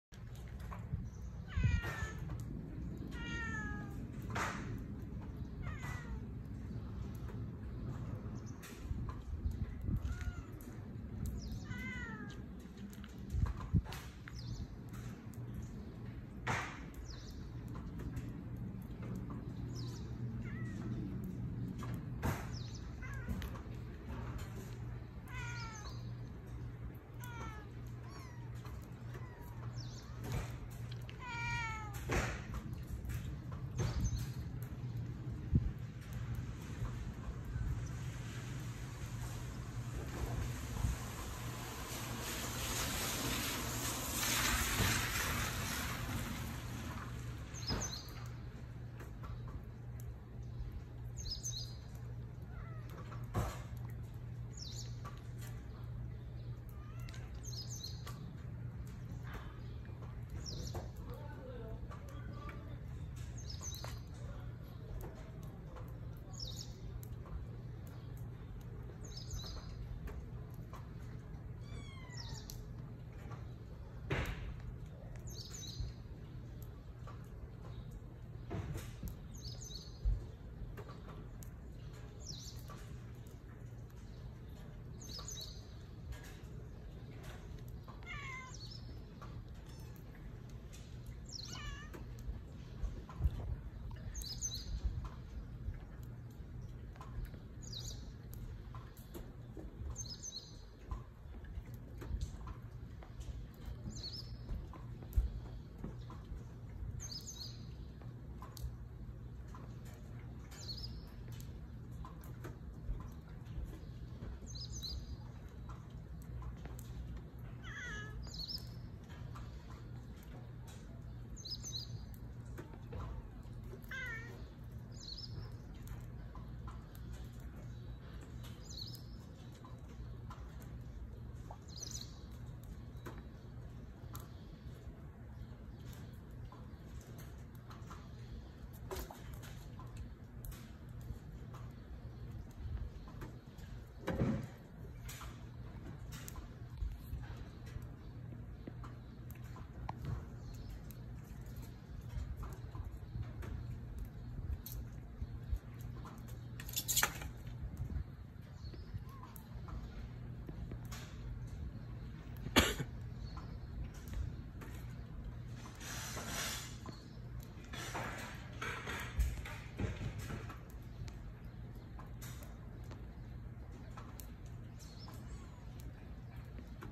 cat baby enjoying with their mother /Catbabysound //babykittens
cats and dogs come from other places in search of food. I think that animals are innocent. thye need to showing love Animals also have the right to live.  I give whatever is in the house, milk, bread, meat and food then they are eats enjoying my heart also fullfield